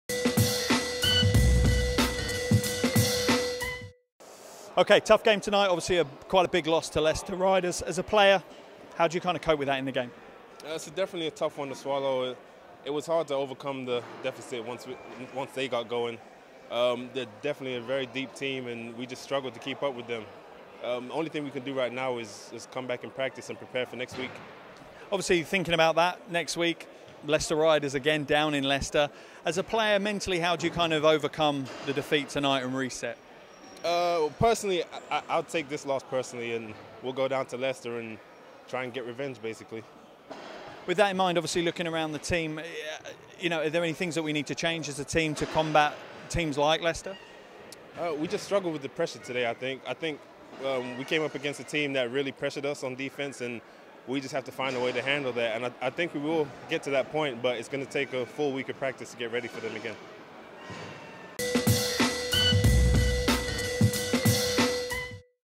The Lowdown with Clayfell Harris
Clayfell Harris sums up the defeat for Leeds Force against the Leicester Riders